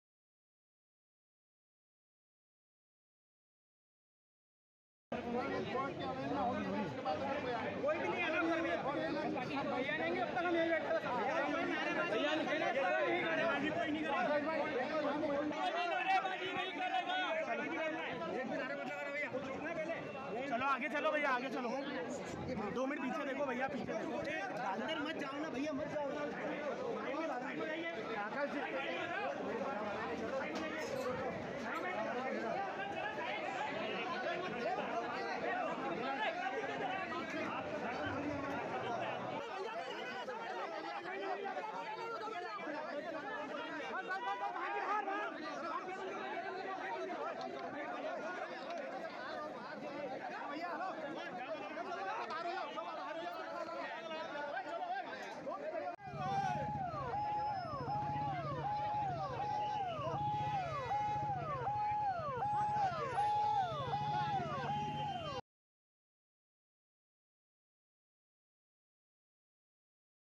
Kailash Vijayvargiya son Akash taken to court for thrashing municipal employee
Indore (MP), June 26 (ANI): Son of Bharatiya Janata party’s senior leader Kailash Vijayvargiya, Akash was taken to court in Indore. Akash Vijayvargiya is also a party MLA from Madhya Pradesh’s Indore. He has been arrested for assaulting a municipal corporation officer by cricket bat on Wednesday.